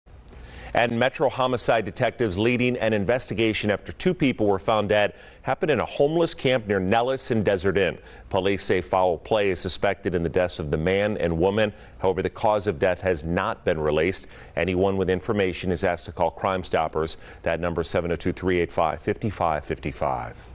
2 people found dead behind east Las Vegas valley business, foul play suspected
Two people were found dead behind an AutoZone business on the east side of the Las Vegas valley on Monday night.